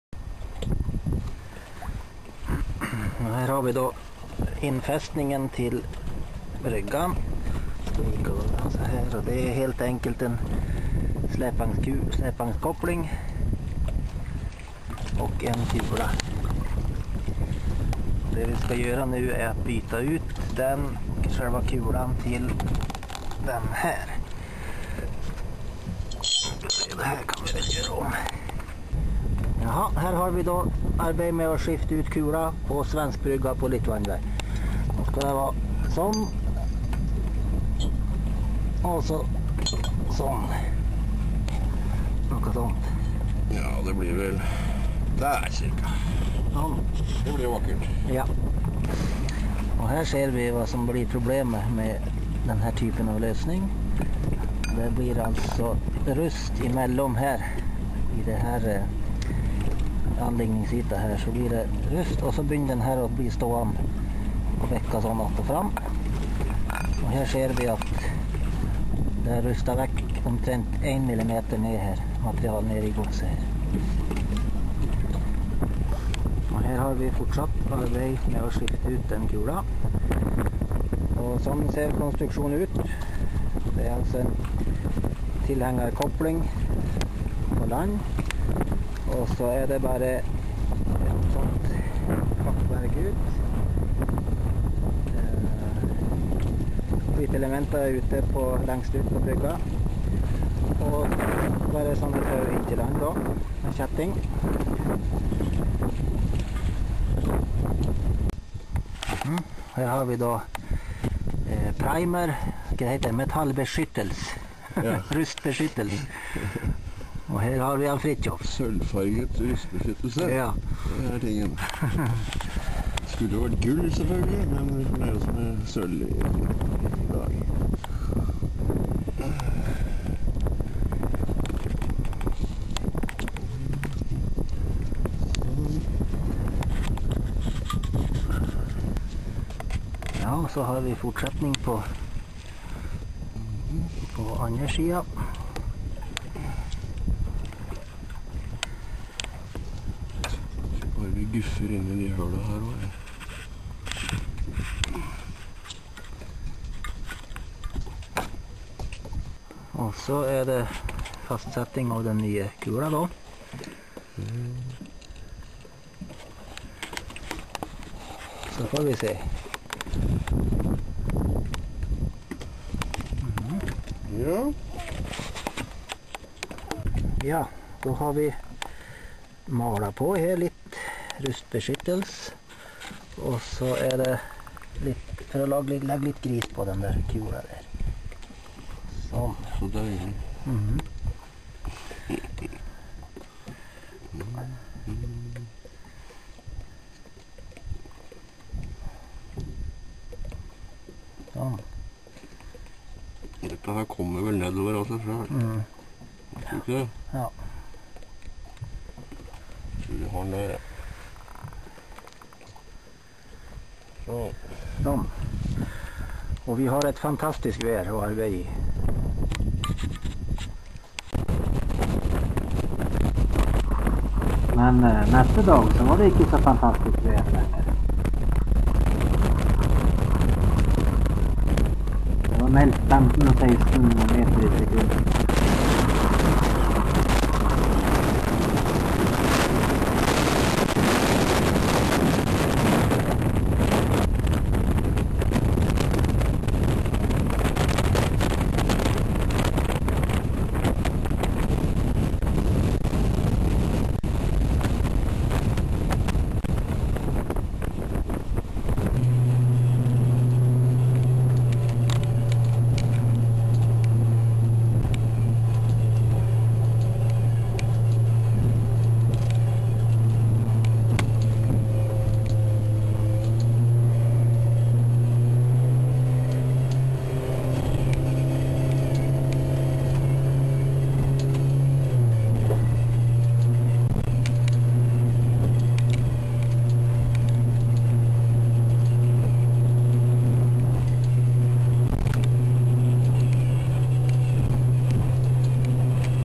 Svenskbrygga på Lille Vandve
På ön Lille Vandve ute på Helgelandskusten finns en flytbrygga konstruerad av Fridtjof Ås efter ritning av Jan-Egil Hemfjäll. Bryggan har legat ute året runt sedan sommaren 2004 och har visat sig vara ett lyckat experiment. Den har klarat vinterstormarna utan problem och endast ett fåtal modifikationer har gjorts.
Denna film handlar om bytet av släpvagnskulan som fungerar som landfäste.